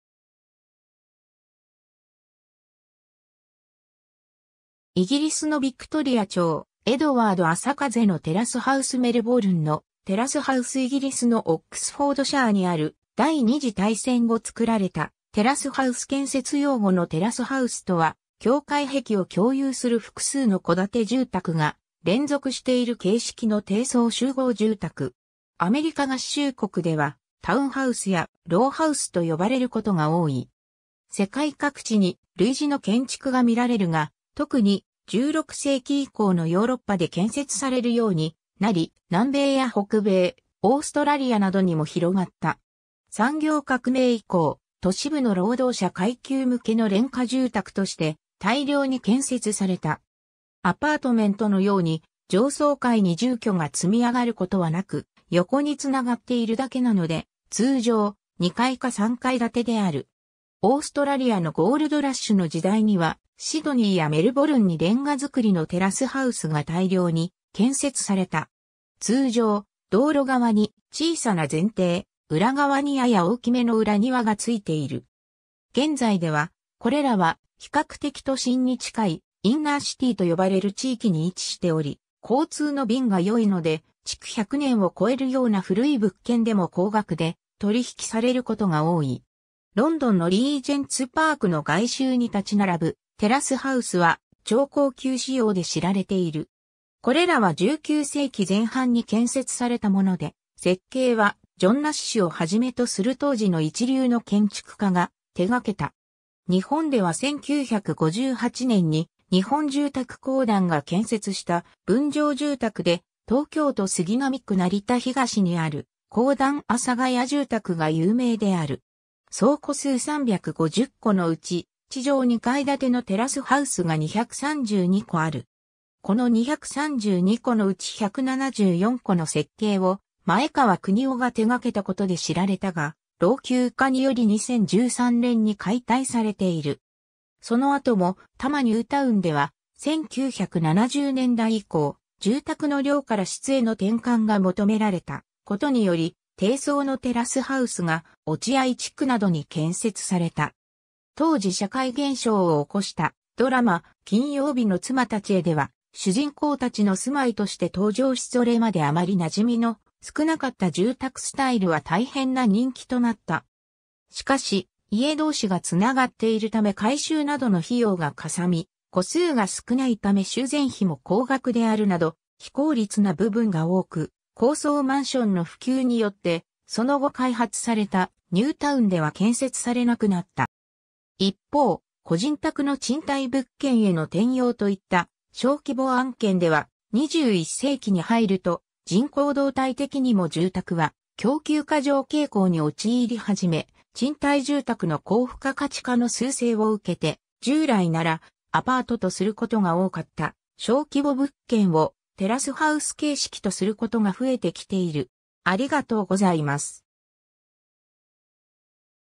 テラスハウス
イギリスのビクトリア朝・エドワード朝風のテラスハウス メルボルンのテラスハウス イギリスのオックスフォードシャーにある第二次大戦後作られたテラスハウス 建設用語のテラスハウス（terraced house）とは、境界壁を共有する複数の戸建て住宅が連続している形式の低層集合住宅。
アメリカ合衆国ではtown house（タウンハウス）やロウハウス（row house）と呼ばれることが多い。
 世界各地に類似の建築が見られるが、特に、16世紀以降のヨーロッパで建設されるようになり、南米や北米、オーストラリアなどにも広がった。
産業革命以降、都市部の労働者階級向けの廉価住宅として大量に建設された。
アパートメントのように上層階に住居が積みあがることはなく、横に繋がっているだけなので、通常、２階か３階建てである。
 オーストラリアのゴールドラッシュの時代にはシドニーやメルボルンにレンガ造のテラスハウスが大量に建設された。
通常、道路側に小さな前庭（フロントヤード）、裏側にやや大きめの裏庭（バックヤード）が付いている。
現在では、これらは比較的都心に近い「Inner city」と呼ばれる地域に位置しており、交通の便が良いので、築100年を超えるような古い物件でも高額で取引されることが多い。
 ロンドンのリージェンツ・パークの外周に建ち並ぶテラスハウスは超高級仕様で知られている。
これらは19世紀前半に建設されたもので、設計はジョン・ナッシュを始めとする当時の一流の建築家が手掛けた。
 日本では1958年（昭和33年）に日本住宅公団が建設した分譲住宅で東京都杉並区成田東にある公団阿佐ヶ谷住宅が有名である。
総戸数350戸のうち、地上2階建てのテラスハウスが232戸ある。
この232戸のうち174戸の設計を前川國男が手がけたことで知られたが、老朽化により2013年(平成25年)に解体されている。
 その後も多摩ニュータウンでは、1970年代以降住宅の"量から質へ"の転換が求められたことにより低層のテラスハウスが落合地区などに建設された。
当時社会現象を起こしたドラマ『金曜日の妻たちへ』では主人公たちの住まいとして登場しそれまであまりなじみの少なかった住宅スタイルは大変な人気となった。
しかし、家同士が繋がっているため改修などの費用がかさみ、戸数が少ないため修繕費も高額であるなど非効率な部分が多く、高層マンションの普及によってその後開発されたニュータウンでは建設されなくなった。
 一方、個人宅の賃貸物件への転用といった小規模案件では、21世紀に入ると人口動態的にも住宅は供給過剰傾向に陥りはじめ、賃貸住宅の高付加価値化の趨勢を受けて、従来ならアパートとすることが多かった小規模物件をテラスハウス形式とすることが増えてきている。